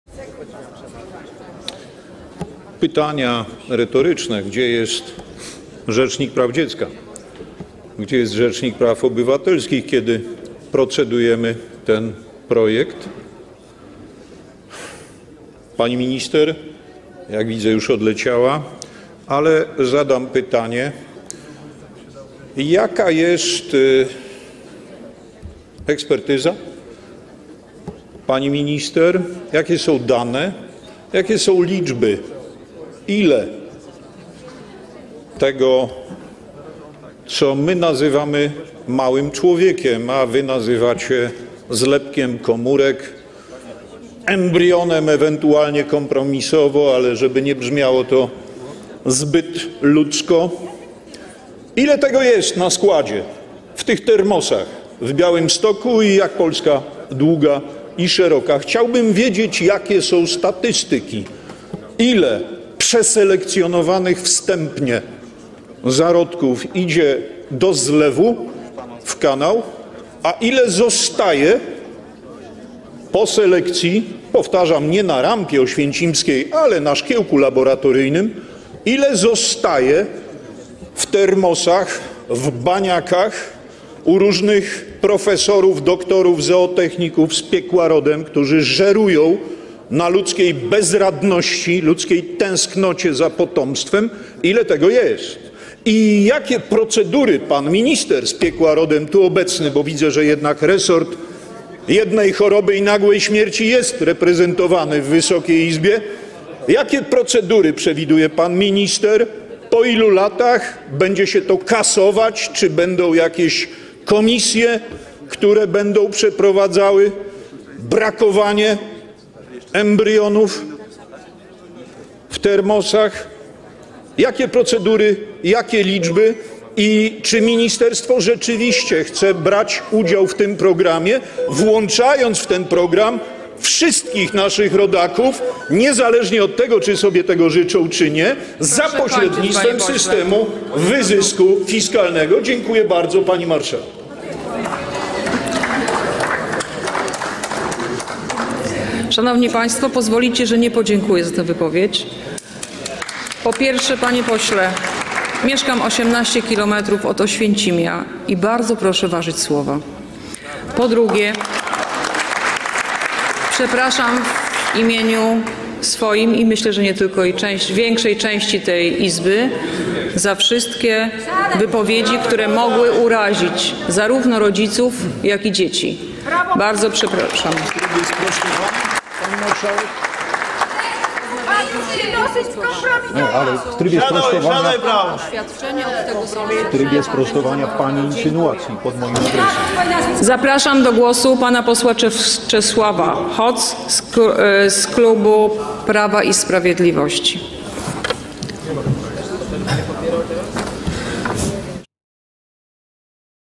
SZOKUJĄCE wystąpienie Brauna w SEJMIE! IN VITRO porównał... do obozu w OŚWIĘCIMIU | FAKT.PL
Na sali sejmowej doszło do sporej awantury. Posłowie zajmują się obywatelskim projektem ustawy ws. refundacji in vitro. — Nie kończy się era dziadersów z Konfederacji — skomentował Joanna Schering-Wielgus z Lewicy. Zaszokowało wystąpienie Grzegorza Brauna.